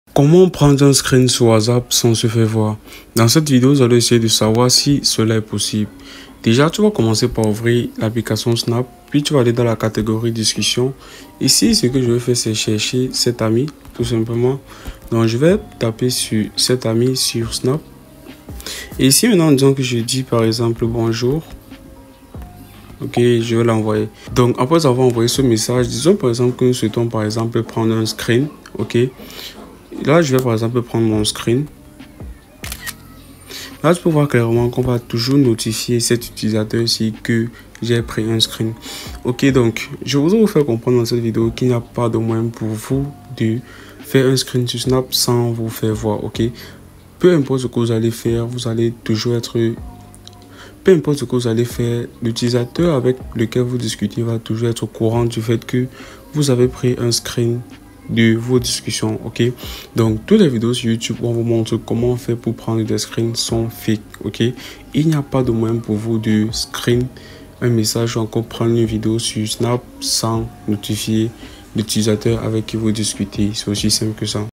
Comment Faire Un Screenshot Sur Snapchat Sans Etre Vu ( 2025 )
Je te montre dans cette vidéo Comment Faire Un Screenshot Sur Snapchat Sans Etre Vu.

A la fin de cette vidéo , tu sauras Comment Faire Un Screenshot Sur Snapchat Sans Etre Vu.

À BIENTÔT 😉